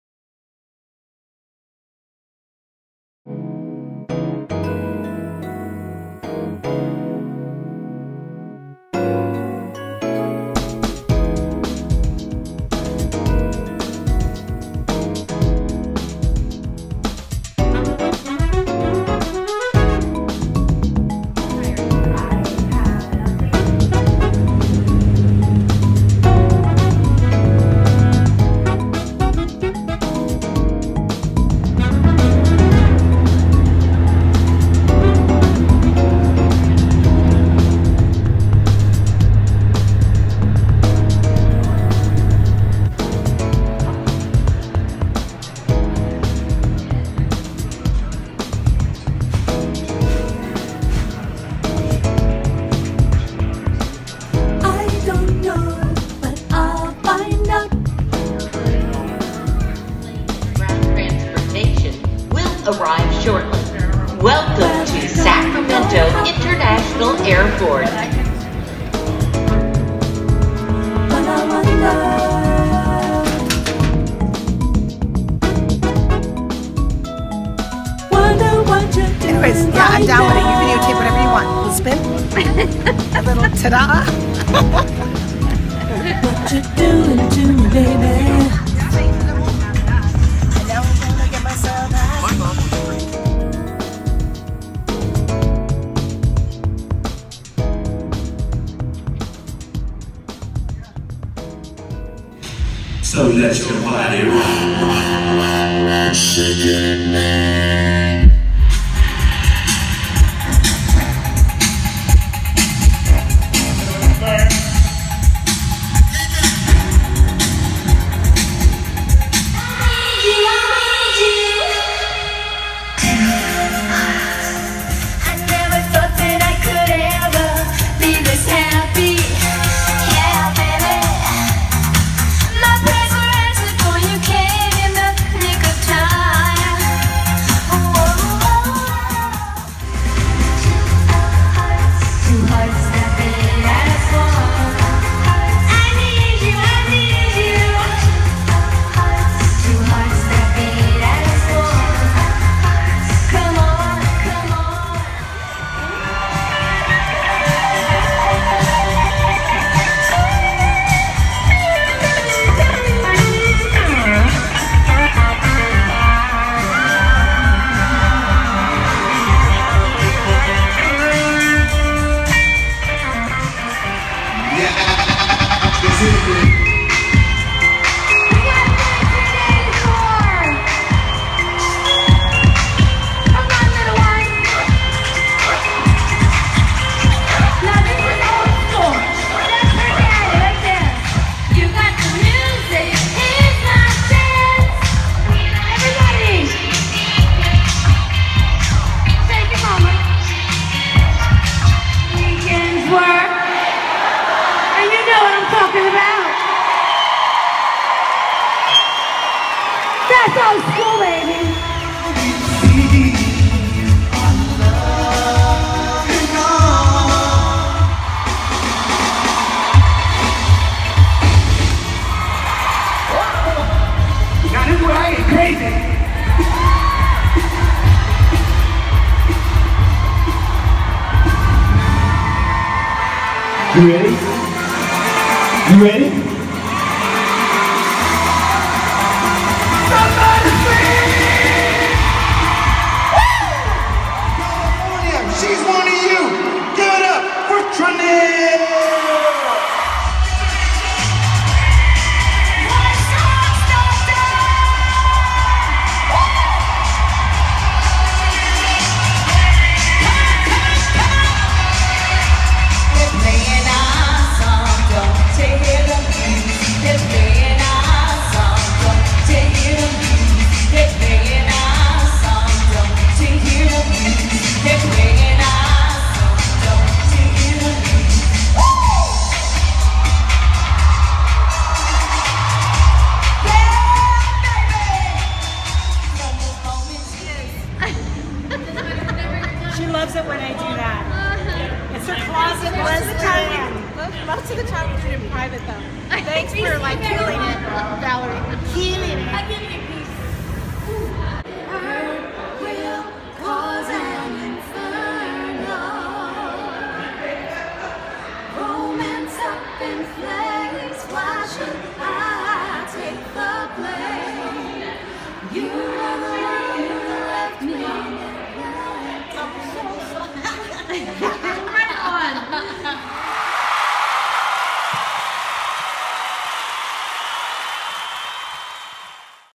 Nu Shooz On The Road: Stockton CA 2015
Nu Shooz On The Road…

Our series of mini movies from the road taken on Valerie’s iPhone.

One of our favorite things about touring is when we get to come into a city a day early and explore. Our other favorite thing is getting to hang out with the acts who are on the shows with us. So we combined the two in these videos to give you a behind the scenes and on-stage look at life on the road with the Shooz.